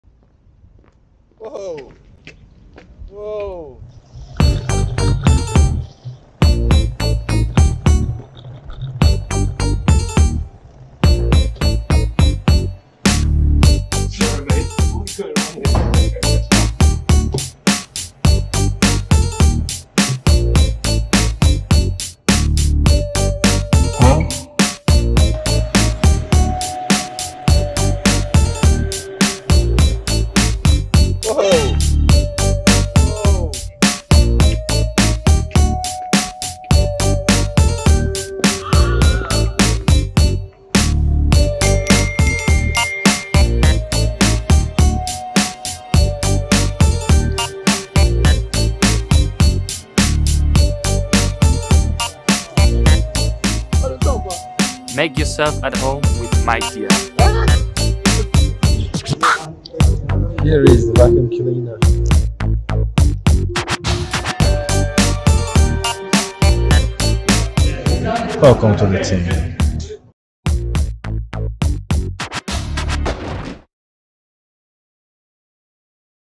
Renovación de nuestra alianza con
Para celebrar la renovación de nuestra alianza con @mancity,  le
enviamos una de nuestras aspiradoras robot a la City Football Academy
para que le dieran un vistazo  y le dijeran: “Make yourself at home”…  🏡

ManchesterCity +